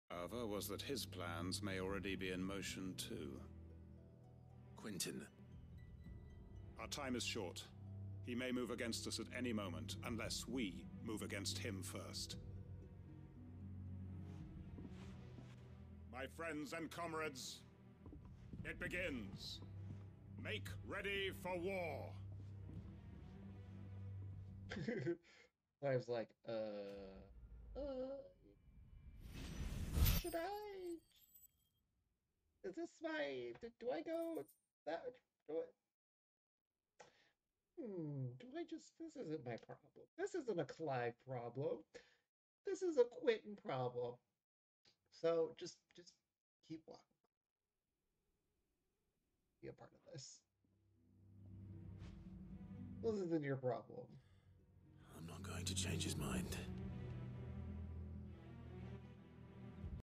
Clive isn’t gonna get involved
I fele like clive is just gonna walk away slowly and be like aint my problem 🤣🤣🤣